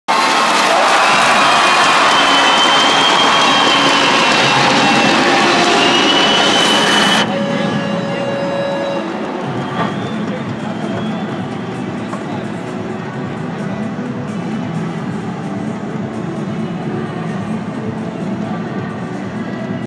Юные хоккеисты играют против ветеранов ЦСКА
20 декабря 2015 года. Сочи. Сборная клуба "Золотая шайба" играет с командой ветеранов ХК ЦСКА, которой руководят двукратные олимпийские чемпионы Борис Михайлов. Встреча закончилась со счётом 14:12 в пользу знаменитостей прошлых лет.